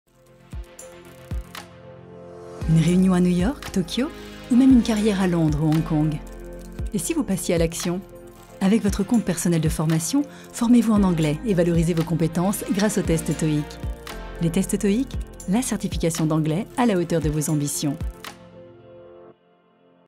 Les tests TOEIC® et le CPF
Avec votre Compte Personnel de Formation, formez-vous en anglais et valorisez vos compétences grâce aux tests TOEIC®.